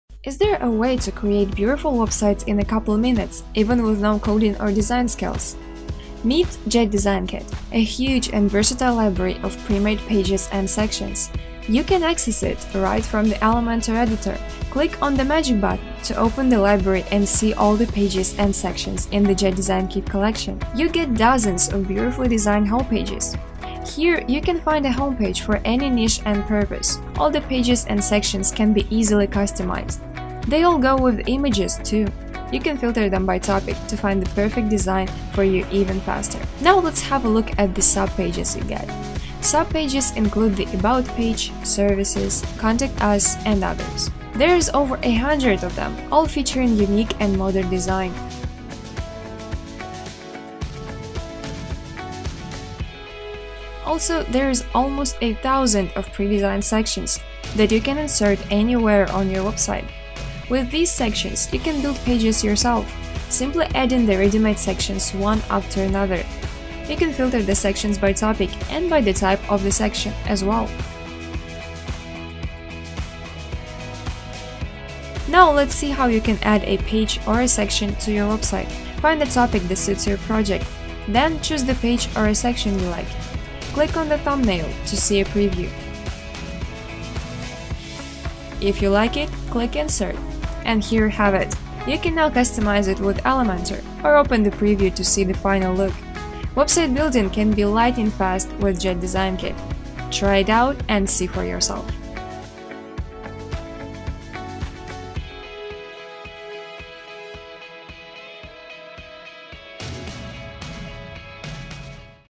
JetDesignKit. Introduction
Meet the brand new JetDesignKit plugin for Elementor, made for creating stunning page templates within seconds!

The new plugin includes +1000 section templates, 200 unique page templates and lots of powerful home pages, providing an easy-to-use Magic Button functionality.

Feel free to get JetDesignKit exclusively at Codecanyon for FREE inside the JetElements plugin's package